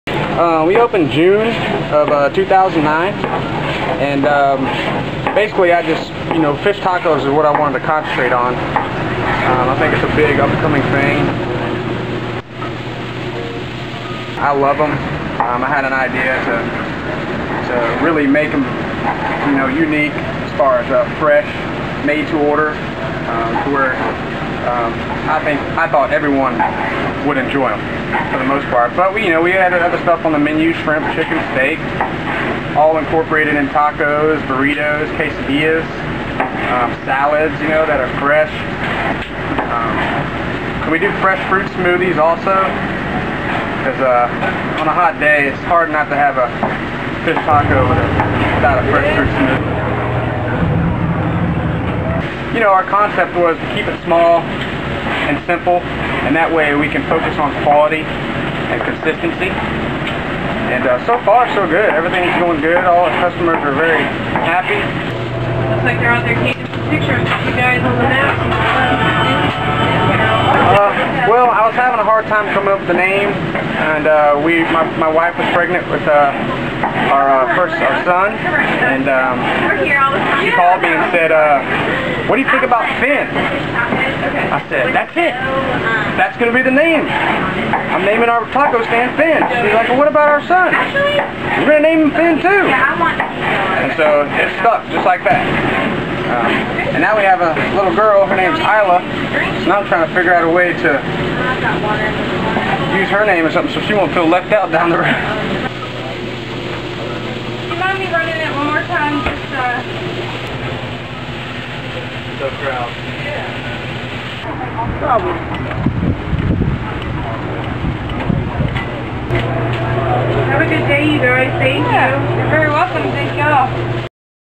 Finn's Island Style Grub
Visit the Panama City Beach food truck, Finn's Island Style Grub, for great ceviches and fish tacos.